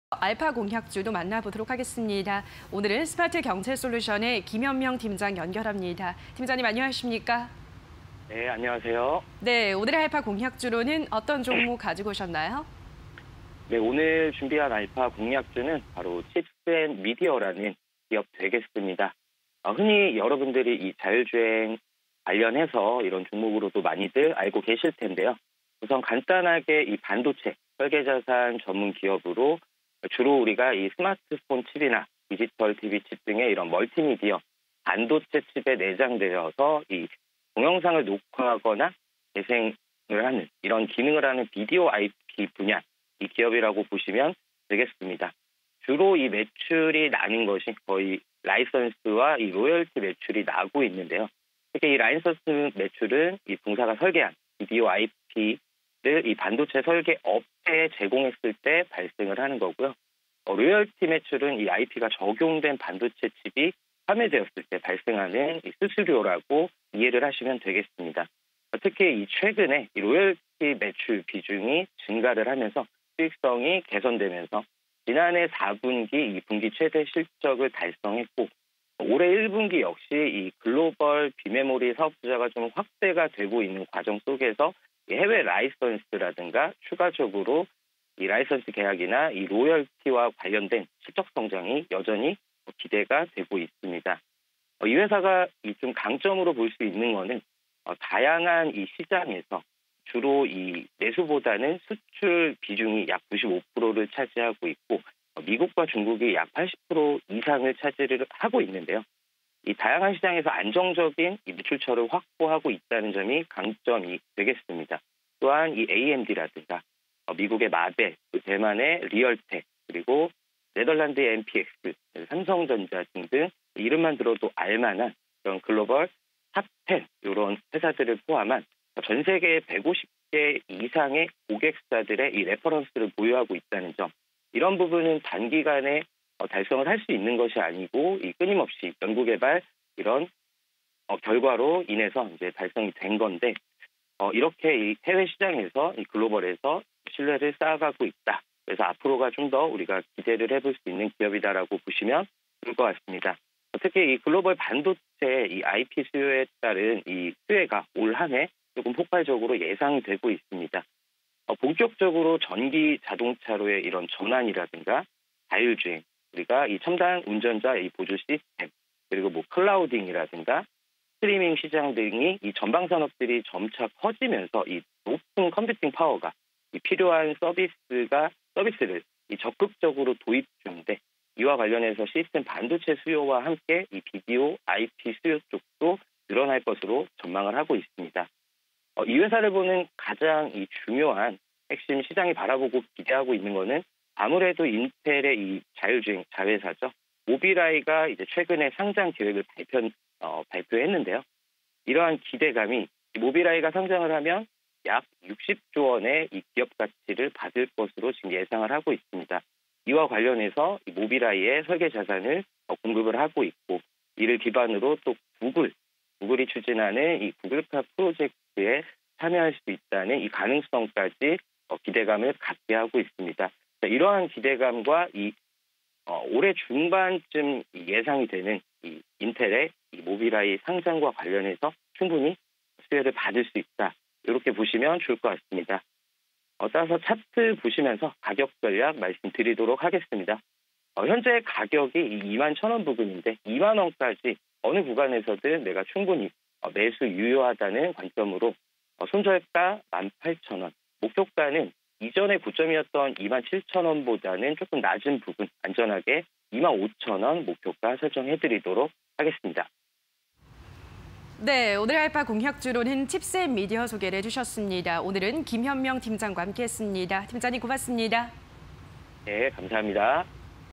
칩스앤미디어(094360)... 로열티 매출 비중 증가로 수익성 개선_알파 공략주 (20220311)
칩스앤미디어(094360)
- 로열티 매출 비중 증가로 수익성 개선
- 다양한 시장에서 안정적 매출처 확보
- 글로벌 반도체 IP 수요 변화 수혜
- 연평균 40% 이상 연구 비용 투자
- 인텔 자회사 '모빌아이' IPO 수혜